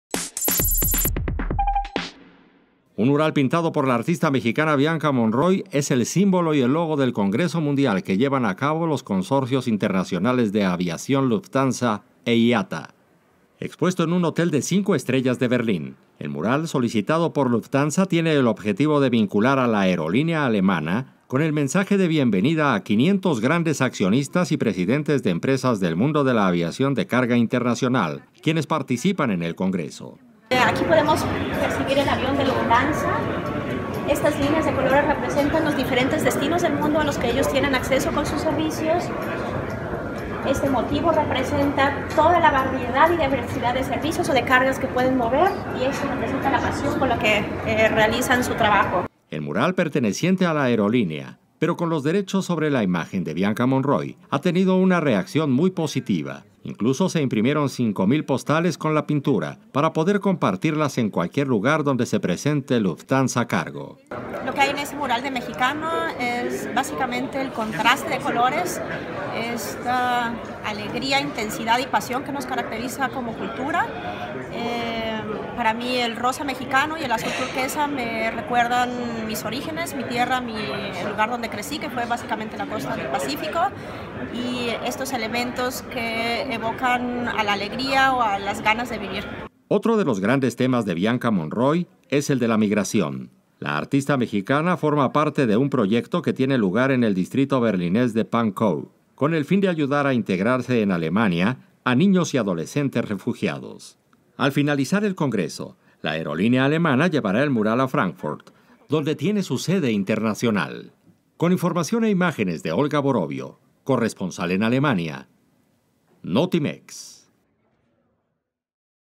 Mural de artista mexicana Bianca Monroy, en Berlín
El mural pintado por Bianca Monroy, artista contemporánea mexicana, es el símbolo y lobo del Congreso mundial de Lufthansa y la Internacional Air Transport Association